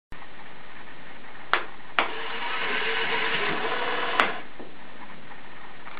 Z scale, R100mm, fast
Prujezd oblouku radius 100mm
Prujezd velkou rychlosti